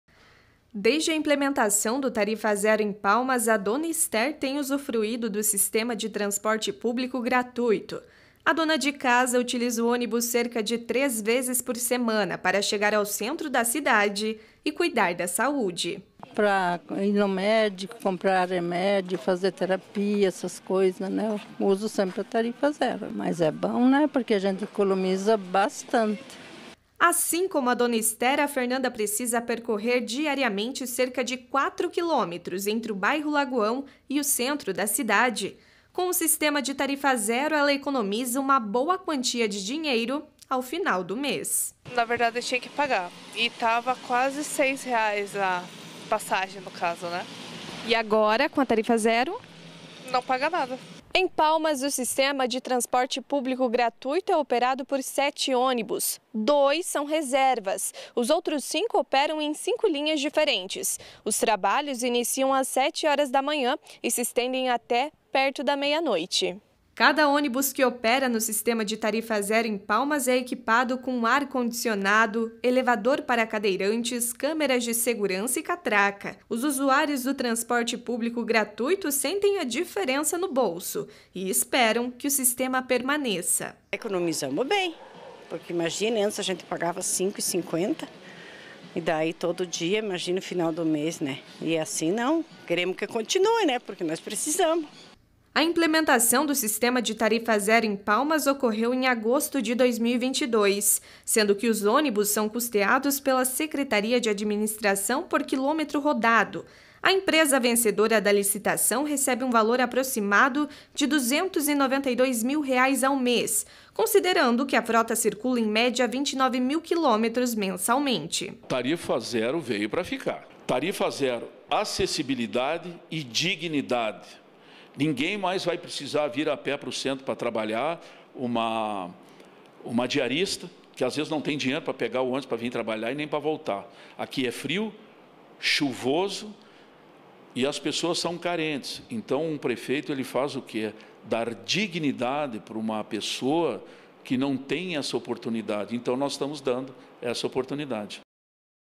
População de Palmas aprova tarifa zero em transporte público
Gestão Pública: Palmas comemora sucesso da tarifa zero nos transportes públicos, proporcionando à comunidade acesso gratuito ao serviço. Cinco linhas são operadas por cinco ônibus no município.